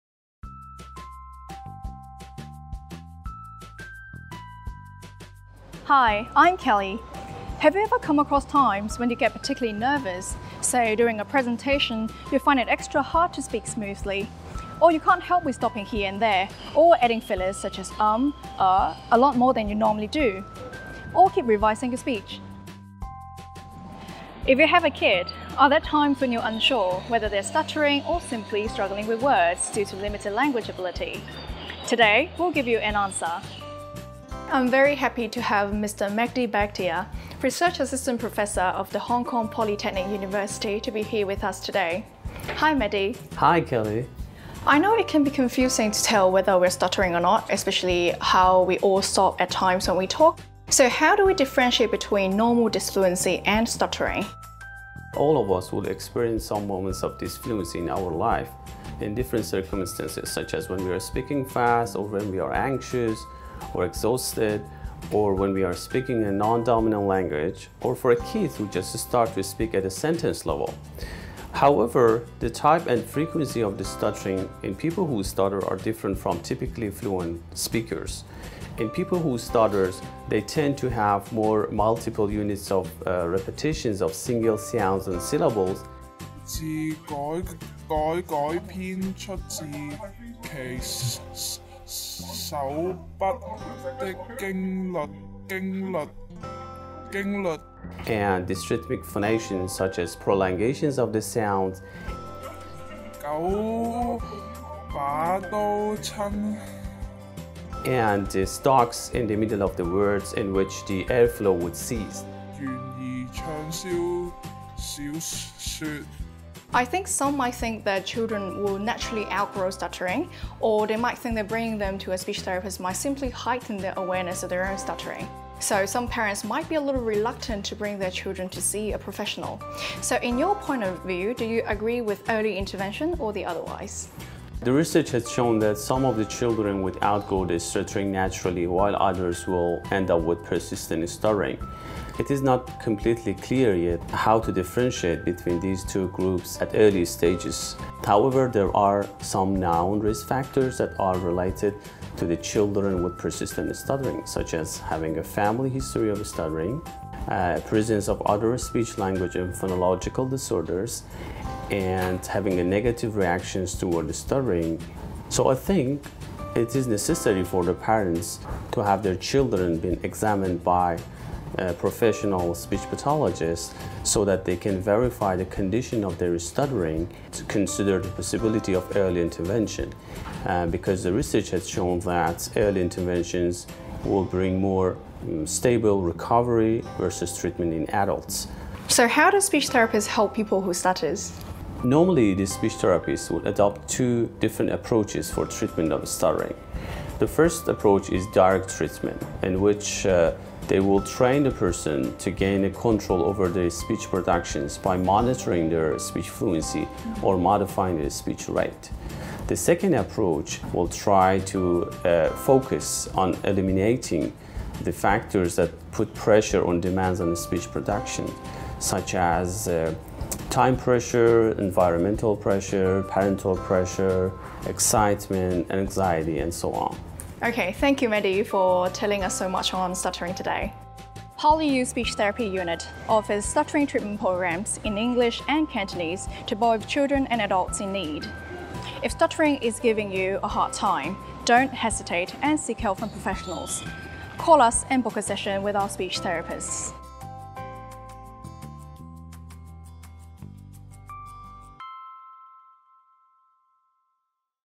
香港理工大學 言語治療所
[口吃]
大家有否試過在某些時間特別緊張，例如在發佈會中，你覺得難以流暢地說話? 其實每一個人說話時也會有停頓的時候，到底我們如何分辨「口吃」和正常的不流暢呢？
孩子到底是口吃還是因語言能力較弱而在找字呢？是否長大後自然會沒有口吃的問題？
言語治療師可以如何幫助受口吃困擾的小孩和成人呢？

[Stuttering]
Have you ever come across times when you get particularly nervous, say during a presentation, you find it extra hard to speak smoothly? Most of us may stop at times when we talk, so how do we differentiate between Normal Dysfluency and Stuttering?
Are kids struggling with words due to language ability or are they stuttering? Will they naturally outgrow stuttering?
How do speech therapists help adults and children who stutter?

查詢方法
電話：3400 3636
Facebook：PolyU Speech Therapy Unit/ 理工大學言語治療所